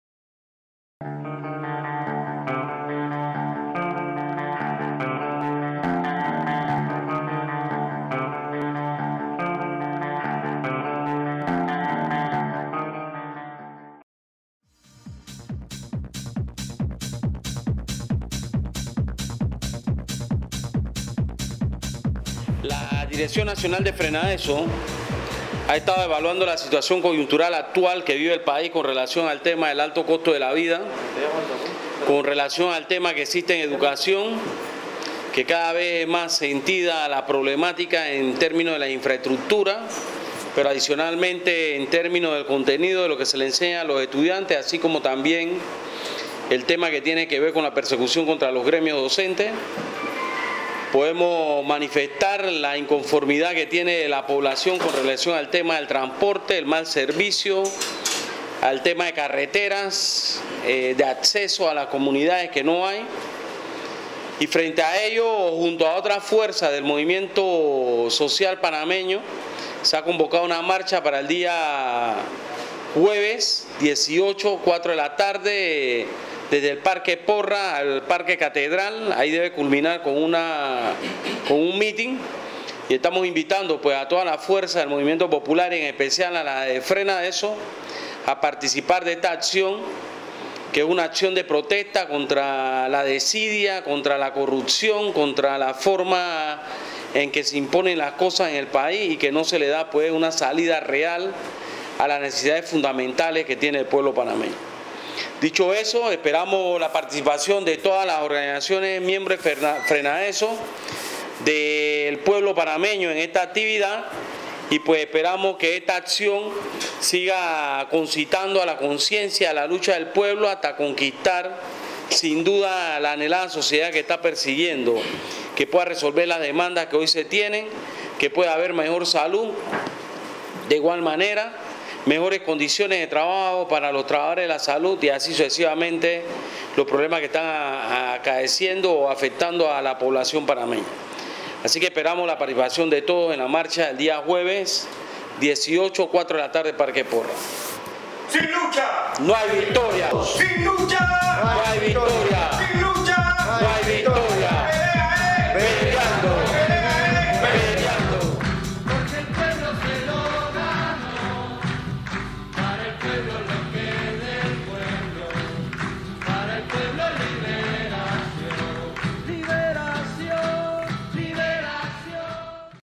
Frenadeso Noticias 16 de Octubre 2012
Conferencia de prensa FRENADESO, participa en la movilización que se anuncia para el jueves 18 de octubre. . .